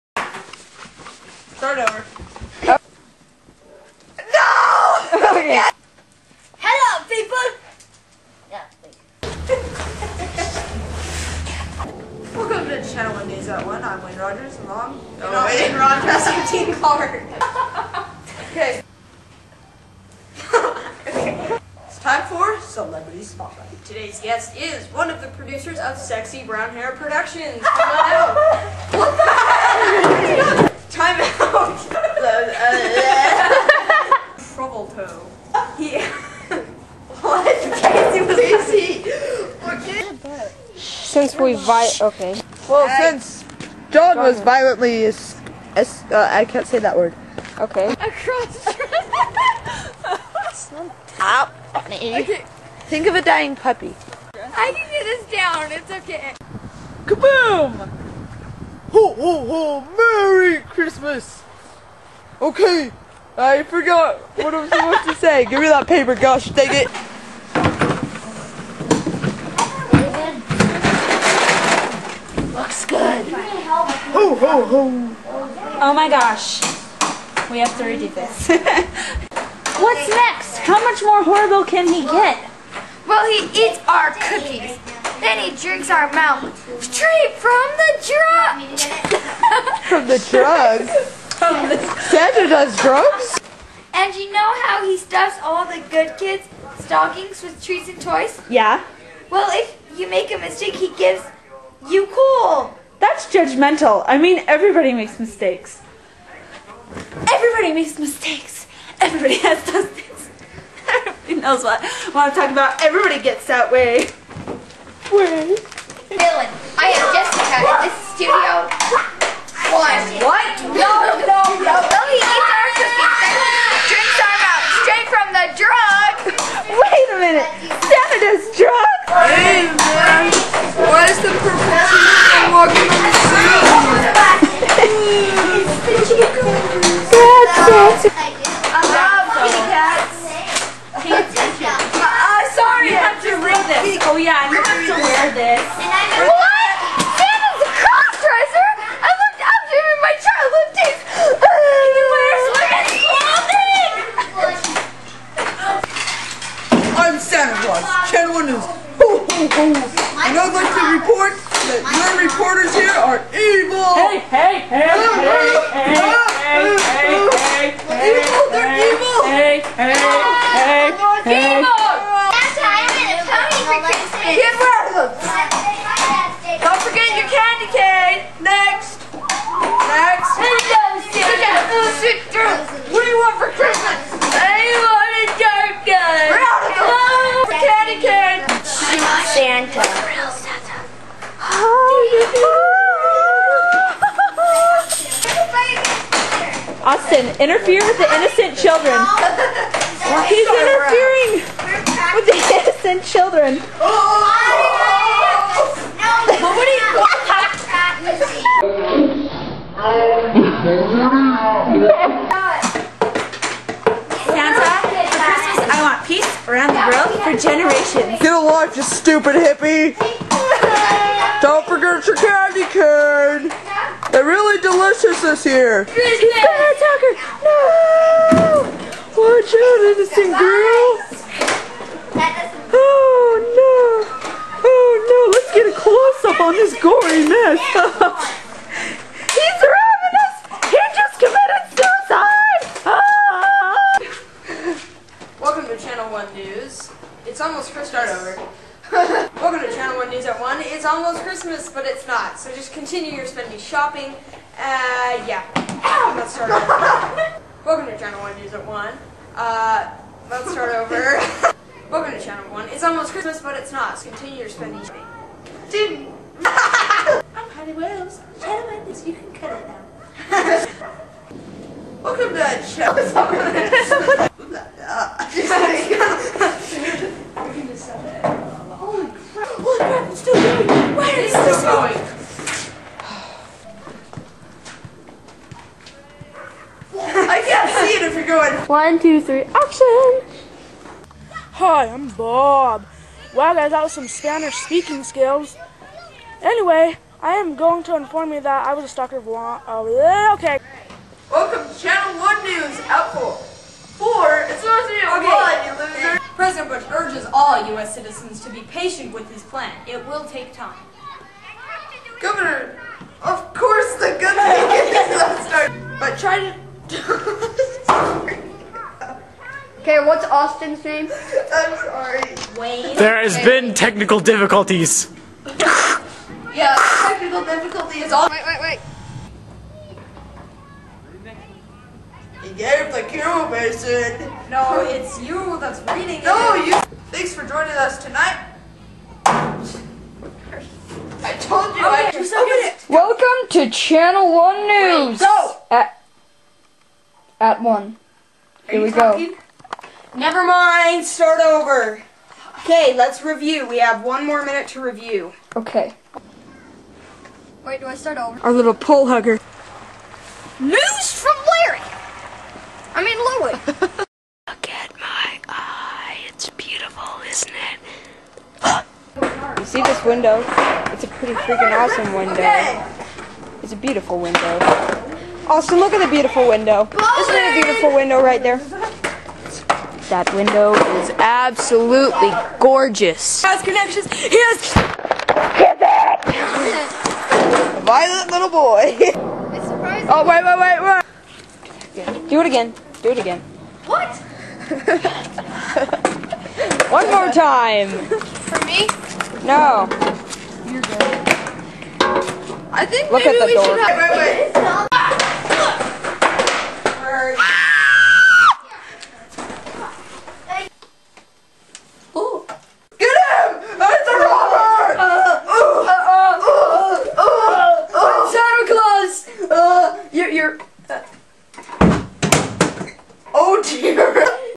Channel 1 News 2008 Bloopers
You've been watching Channel 1 News for awhile now but, have you ever wondered if you're not seeing anything? Well you're not but, now you can! See all the times we've messed up, that you would never see on a newscast.